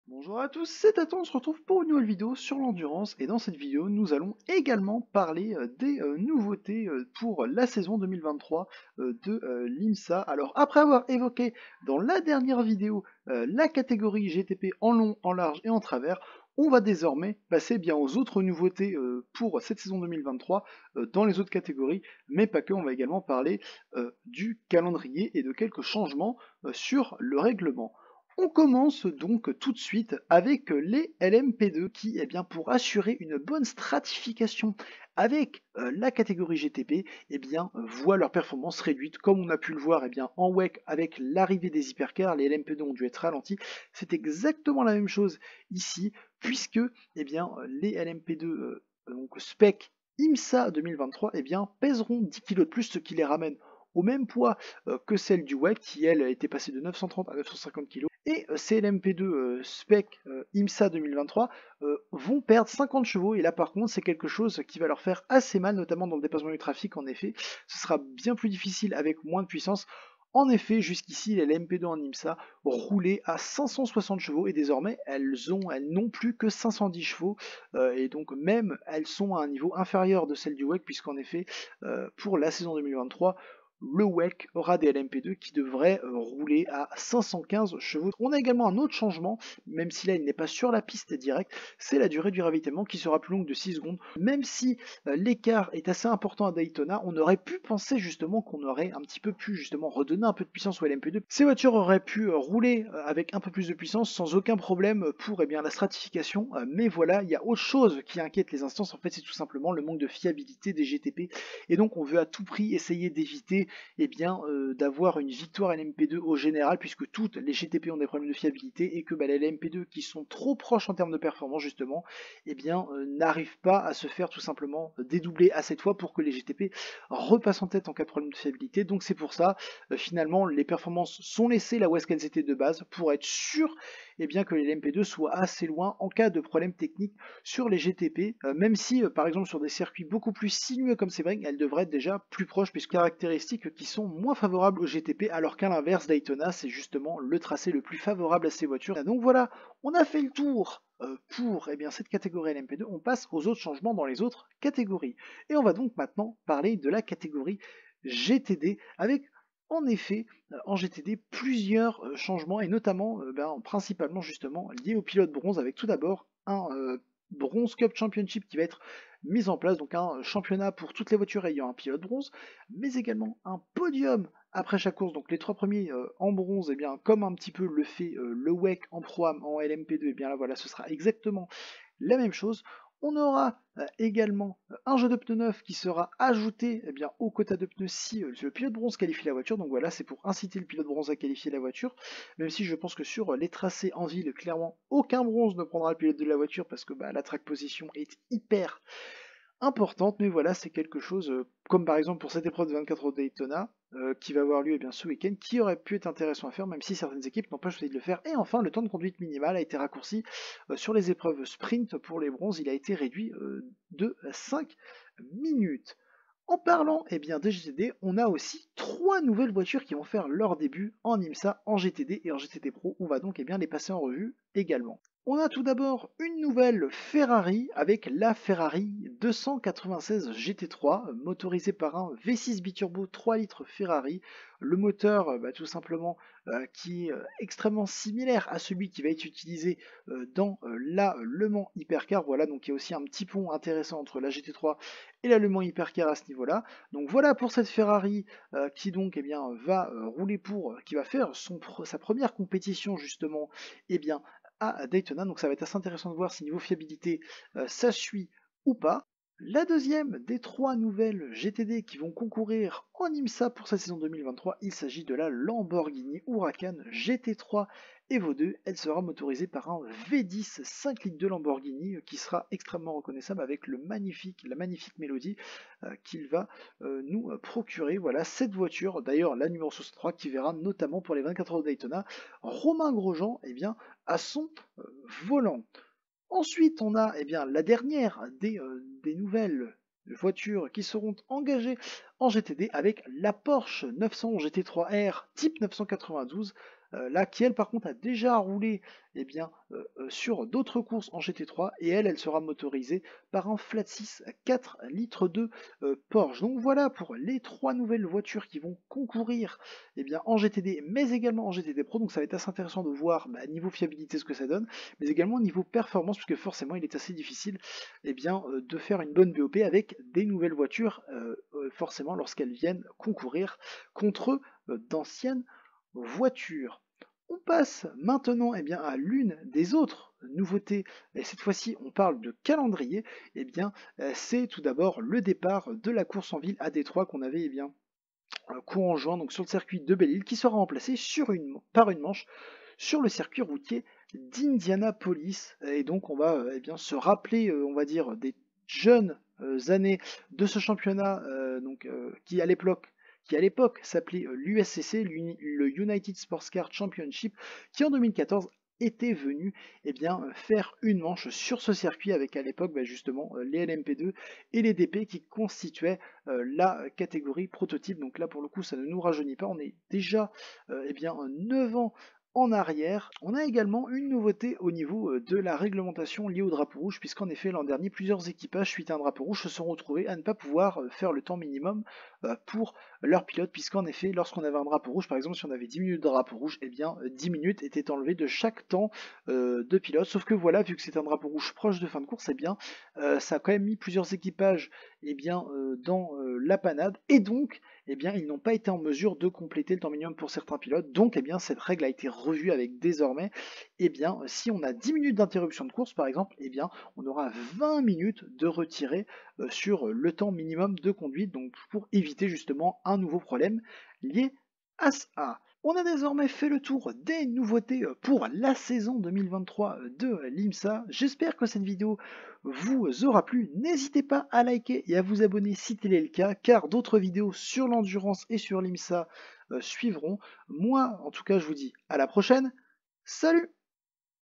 GUIDE IMSA ET 24H DE DAYTONA 2023 #2 LES NOUVEAUTES DANS LES AUTRES CATEGORIES
Bonjour à tous on se retrouve pour une vidéo qui va passer en revue les nouveautés dans les différentes catégories, le règlement et le calendrier pour la saison 2023 d'l'IMSA.